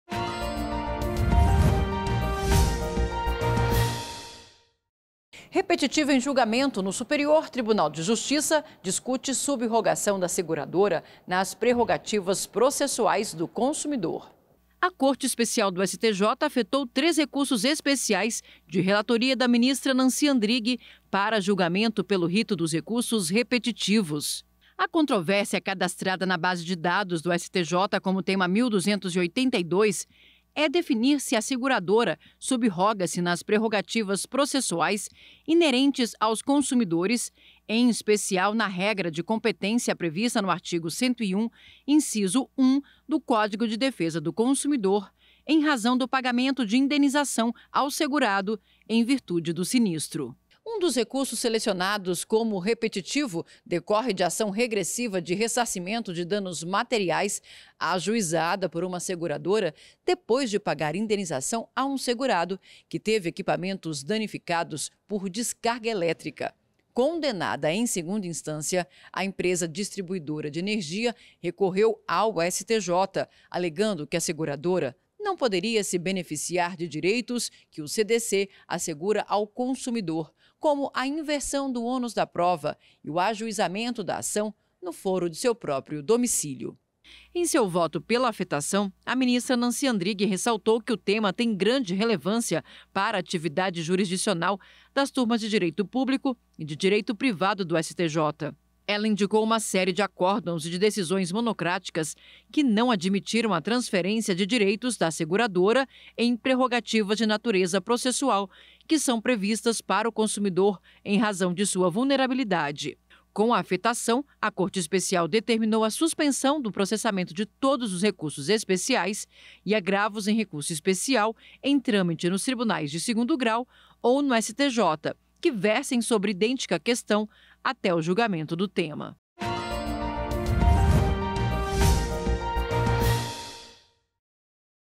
Repetitivo discute sub-rogação da seguradora nas prerrogativas processuais do consumidor
​A Corte Especial do Superior Tribunal de Justiça (STJ) afetou os Recursos Especiais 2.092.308, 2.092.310 e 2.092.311, de relatoria da ministra Nancy Andrighi, para julgamento pelo rito dos recursos repetitivos.
A controvérsia, cadastrada na base de dados do STJ como Tema 1.282, é "definir se a seguradora sub-roga-se nas prerrogativas processuais inerentes aos consumidores, em especial na regra de competência prevista no artigo 101, inciso I, do Código de Defesa do Consumidor (CDC), em razão do pagamento de indenização ao segurado em virtude do sinistro".
REsp 2092308
REsp 2092310
REsp 2092311